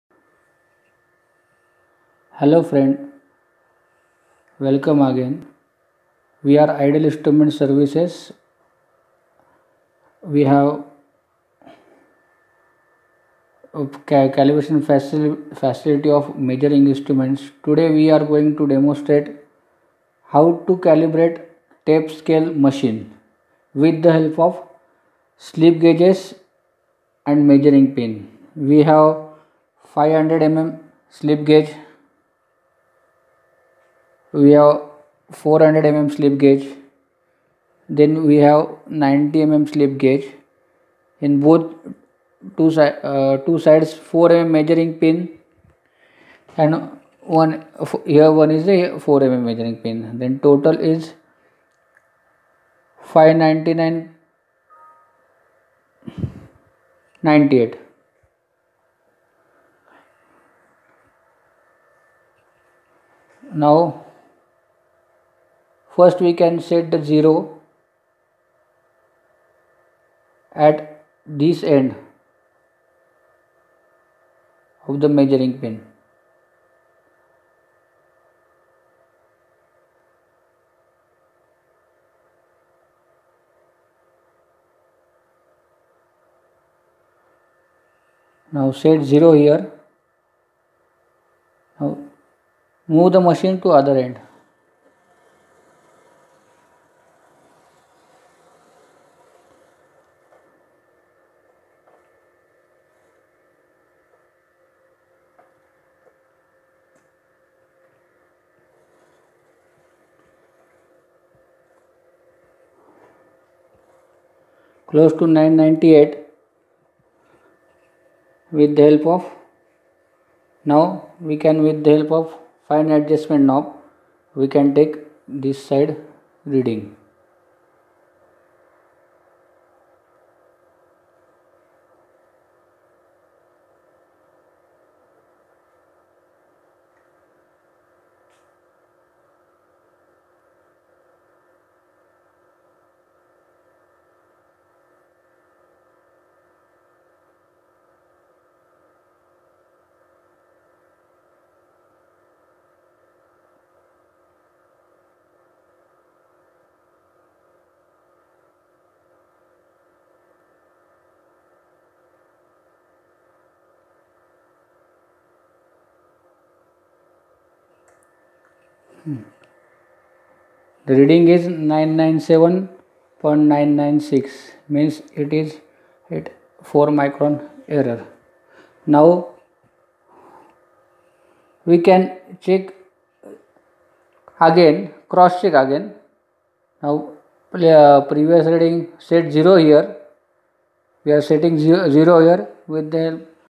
calibration of octogan tape scale machin
calibration procedures for calibration of tape scale machine